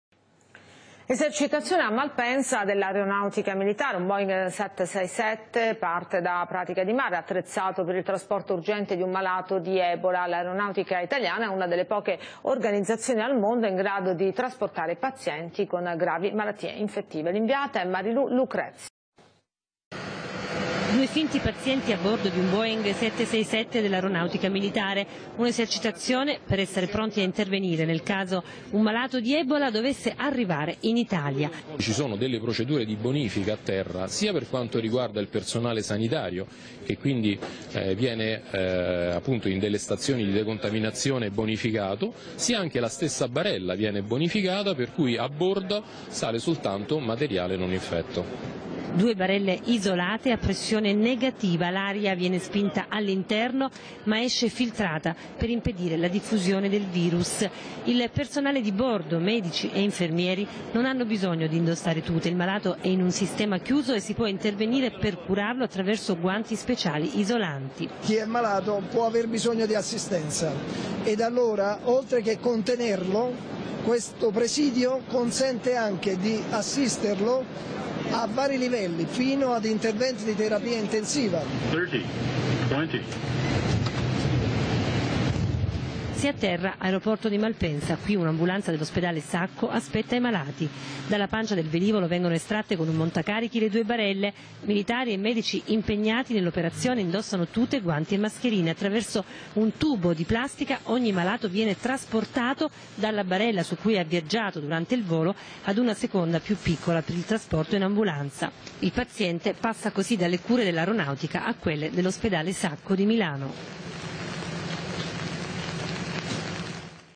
Il Dott. Ferdinando Arganese dirige una esercitazione di biocontenimento
Il Collega Ferdinando Arganese ha diretto le operazioni sanitarie in volo di una esercitazione di biocontenimento di malati altamente contagiosi  simulando la gestione e trasporto aereo  di 2 pazienti altamente infettivi (Ebola)